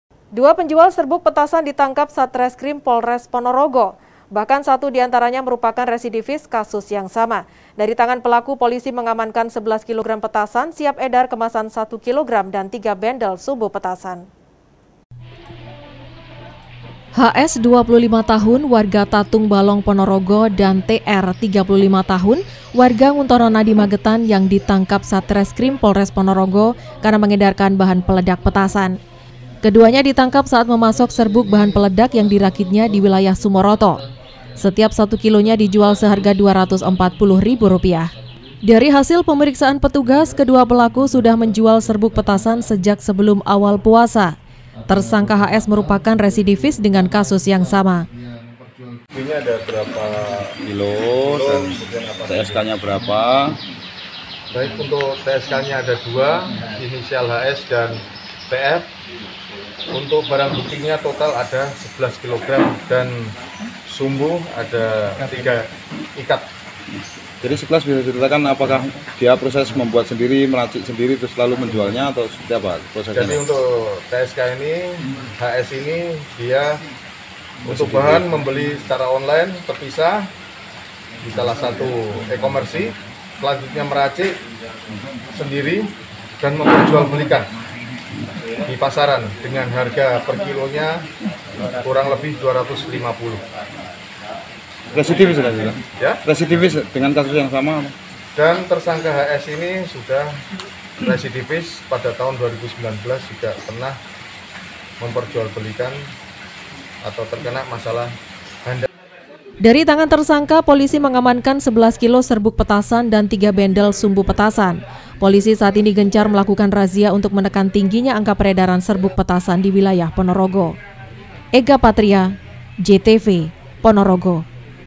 PONOROGO - Simpan 11 KG Serbuk Petasan Siap Edar, 2 Residivis Pengedar Ditangkap
Dua penjual serbuk petasan ditangkap satreskrim polres ponorogo. Bahkan 1 diantaranya merupakan residivis kasus yang sama.  Dari tangan pelaku polisi mengamankam 11 kilogram petasan siap edar kemasan 1 kiloan dan 3 bendel sumbu petasan.